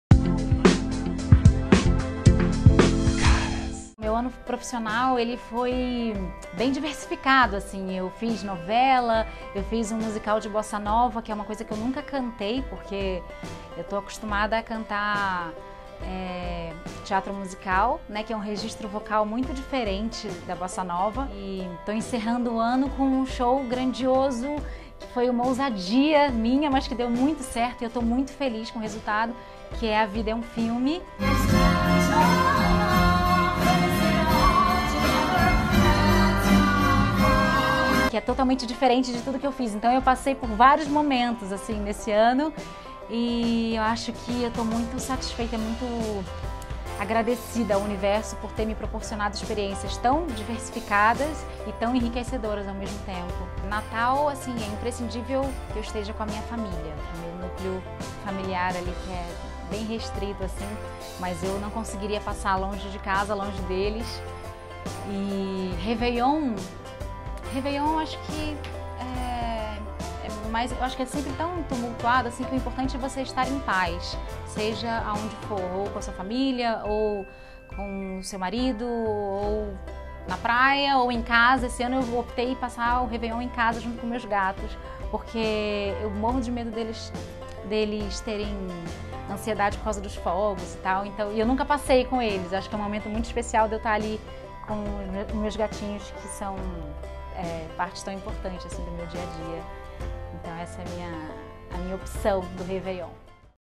Fabi Bang: reveillon com seus gatos
A atriz e cantora Fabi Bang faz um balanço positivo de 2017 e revela que a passará a virada do ano junto de seus gatos! Veja a entrevista.